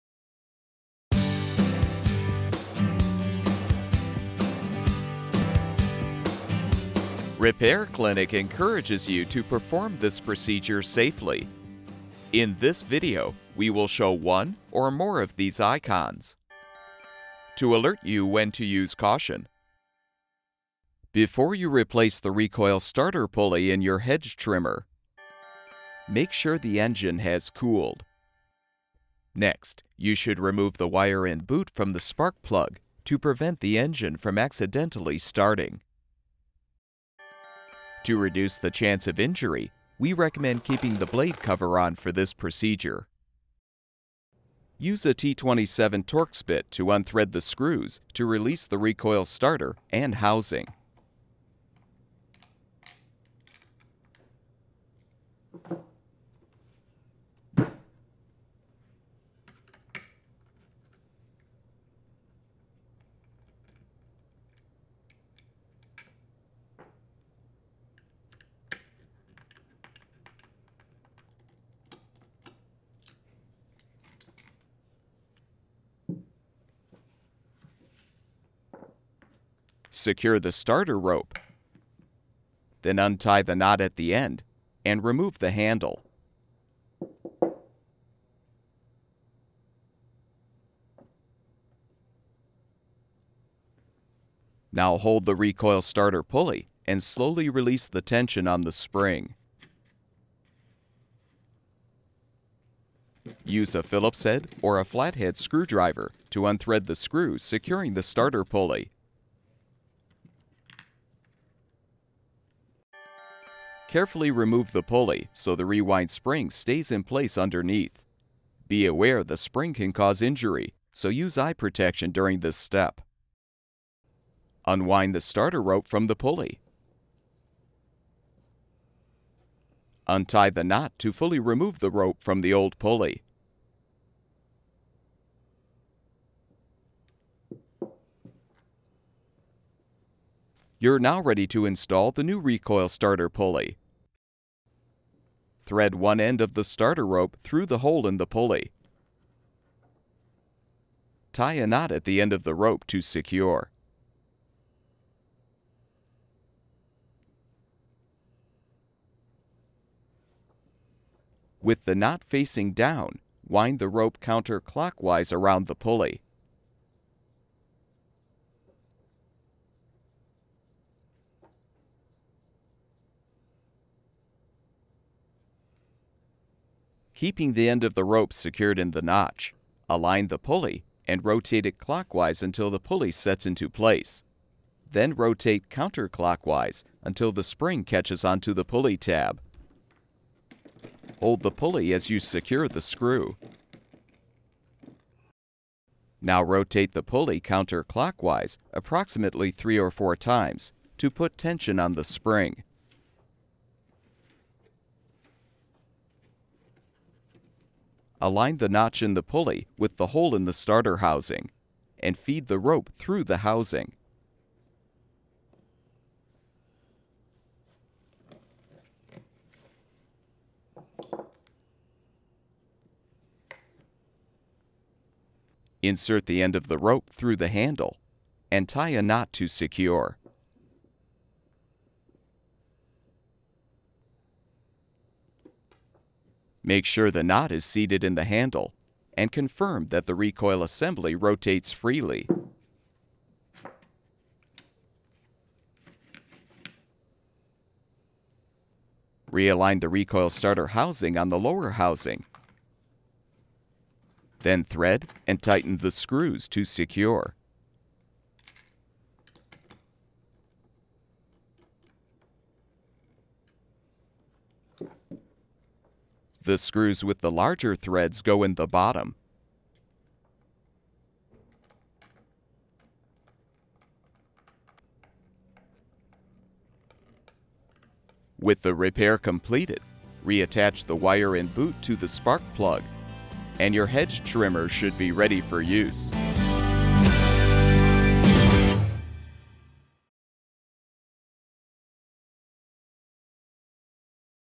Echo Hedge Trimmer Recoil Starter Pulley #17721544430
This video provides step-by-step repair instructions for replacing the recoil starter pulley on an Echo hedge trimmer. The most common reasons for replacing the pulley are when the rope will not rewind or when the pulley itself is broken.

Additional hedge trimmer repair, troubleshooting tips, help finding your model number, and part replacement videos

All of the information in this pulley replacement video is applicable to the following brands: Echo

Tools used: T-27 Torx bit, screwdriver